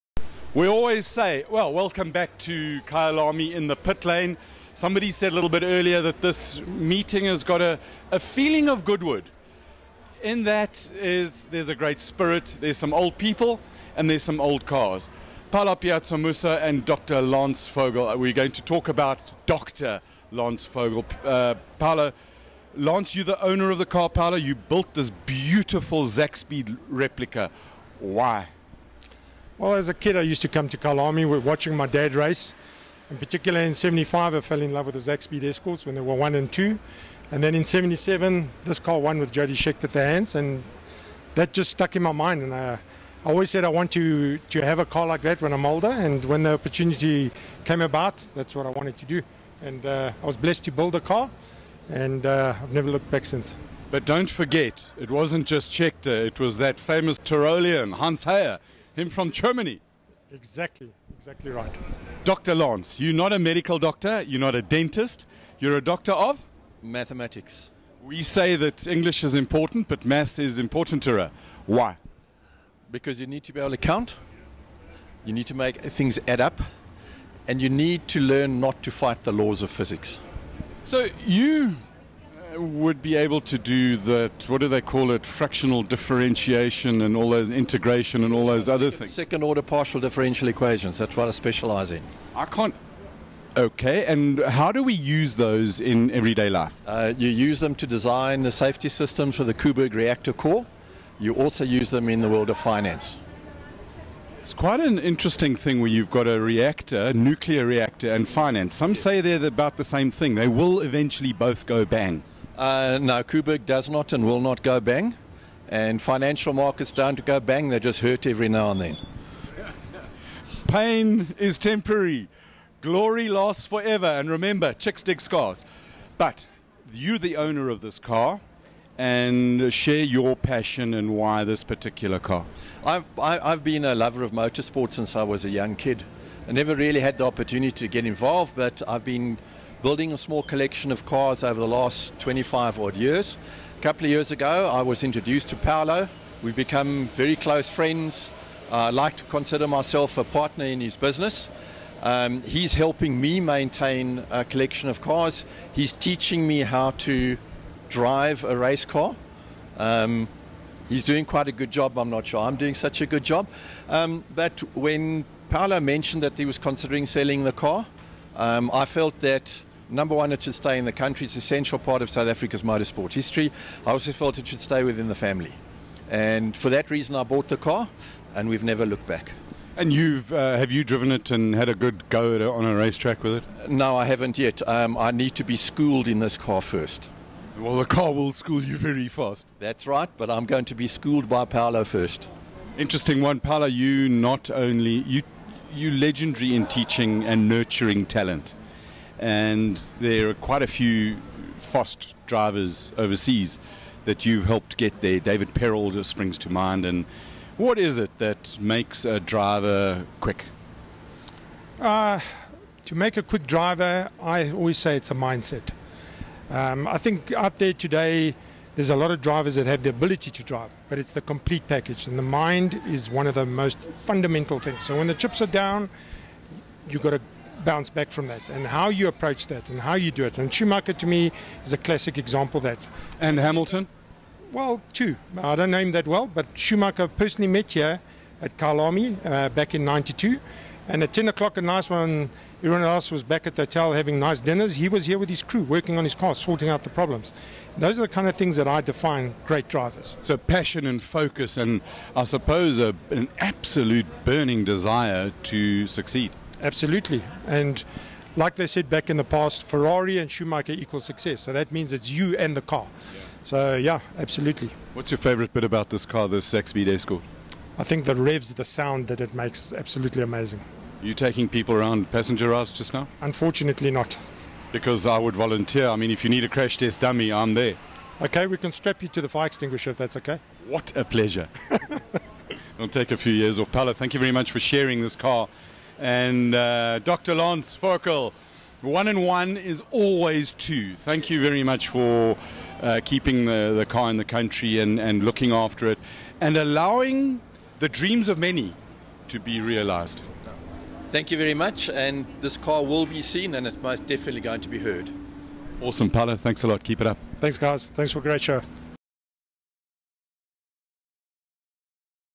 ZakSpeed Replica with Paulo Piazza Musso
We chat to builder of the ZakSpeed replica Ford Escort Paulo Piazza Musso and

Owner Dr Lance Vogel